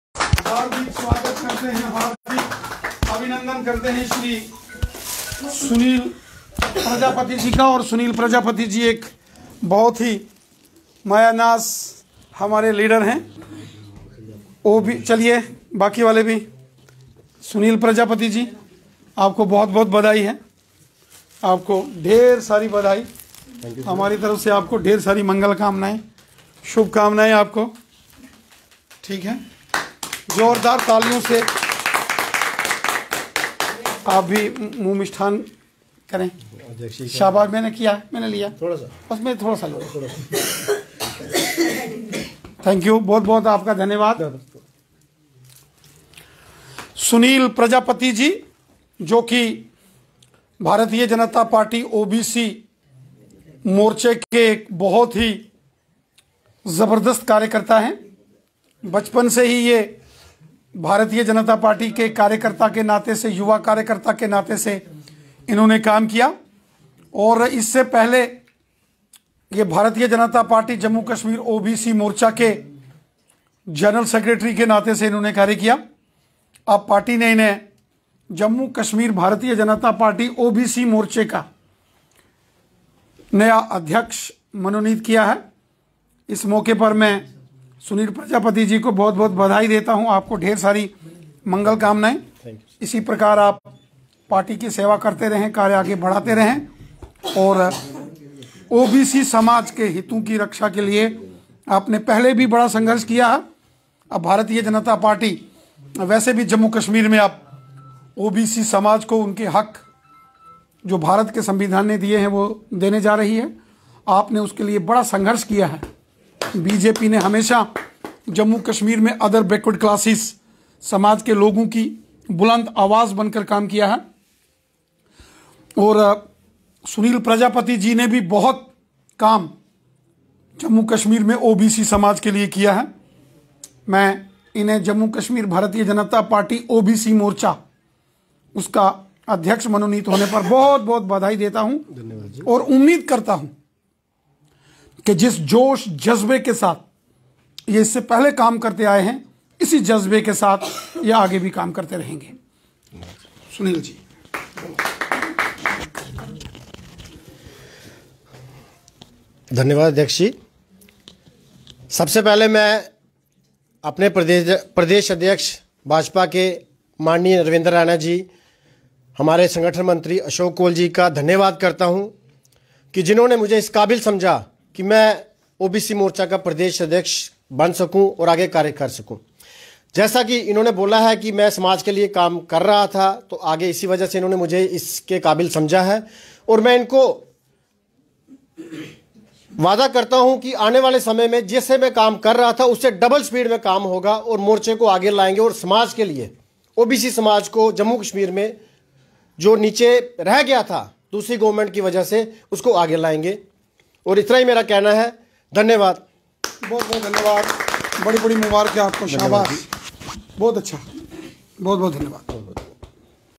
Sh Ravinder Raina felicitated Sh Sunil Prajapati at BJP HQ, Jammu.
J&K BJP President Sh. Ravinder Raina felicitated newly nominated President BJP OBC Morcha J&K Sh. Sunil_Prajapati at BJP HQ, Jammu.